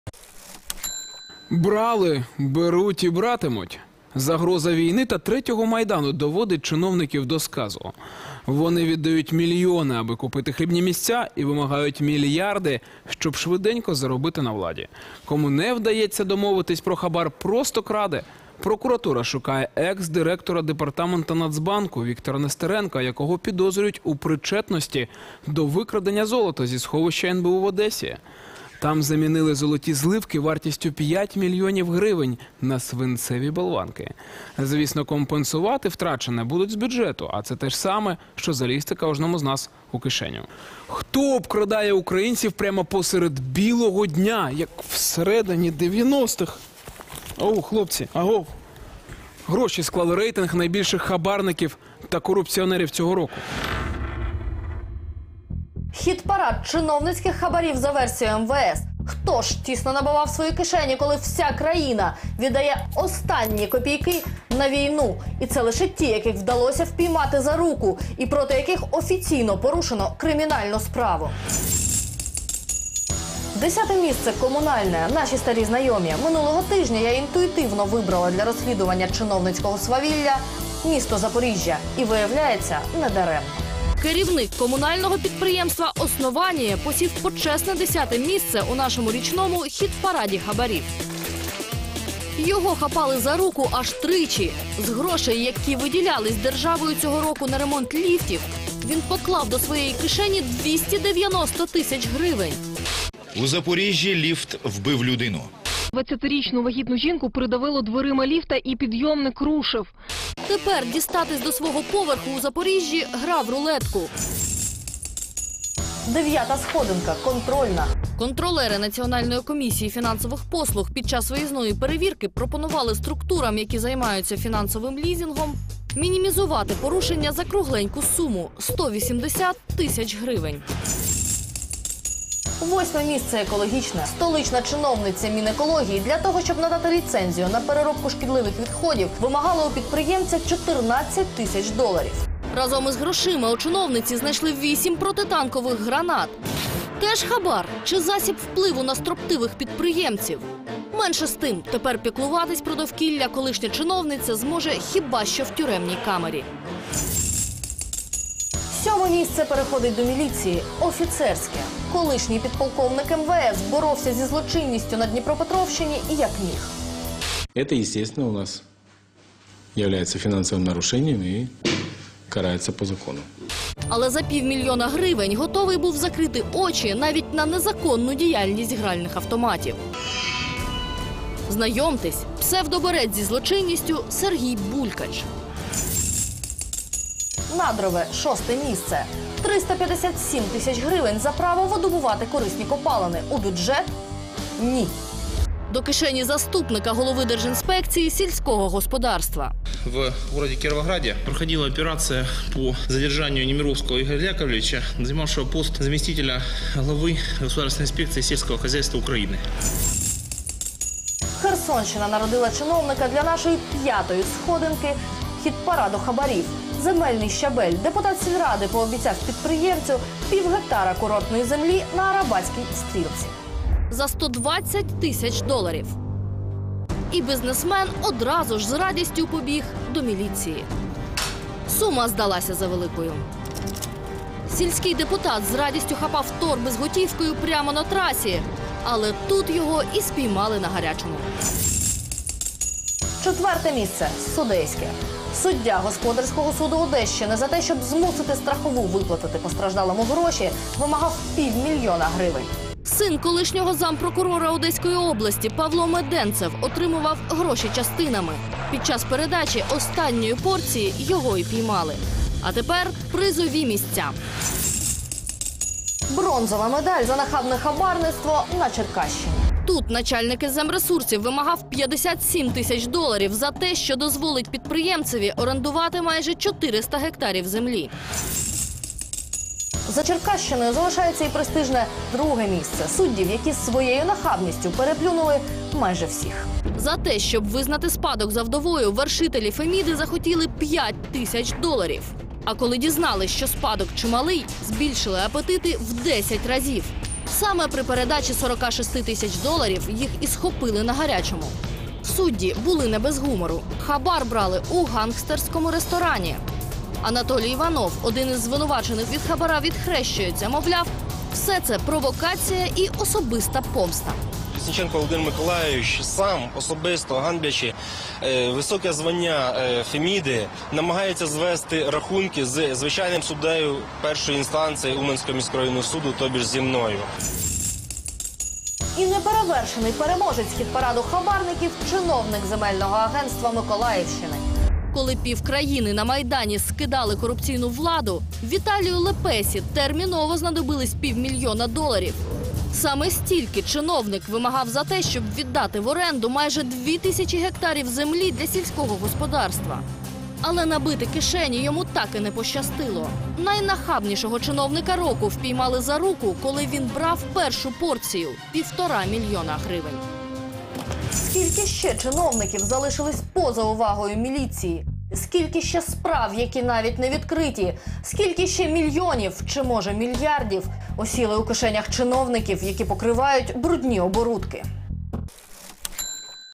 Гроші. Рейтинг хабарників
Хіт-парад чиновницьких хабарів за версією МВС. Хто ж тісно набивав свої кишені, коли вся країна віддає останні копійкИ на війну?